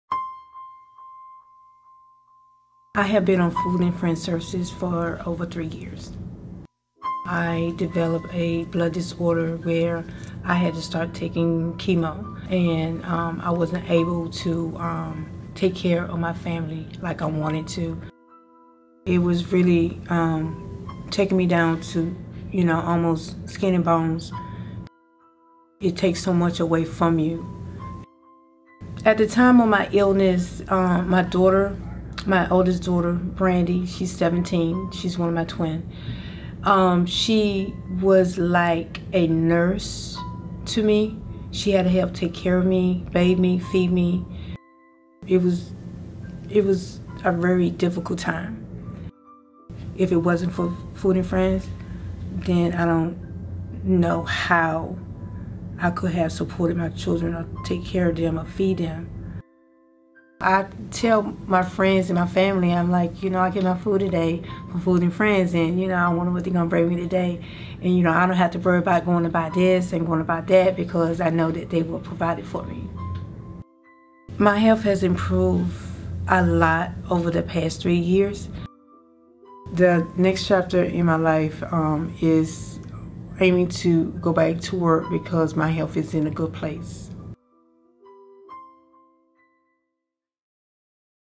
Hope Begins with You - Meet Denise
In 2010, Denise was diagnosed with a rare blood disease that required chemotherapy treatments. With four children, graduate school courses, frequent hospital visits and a lack of income due to job loss, food was a major hurdle in Denise's life.

Four years after starting service with Food & Friends, Denise is now searching for a new job, attending Catholic University and taking cooking classes at Food & Friends' facility. With the help of our frequent meal deliveries, Denise continues to inspire us with her phenomenal story.

Support our Summer Food Fund and help us serve more people like Denise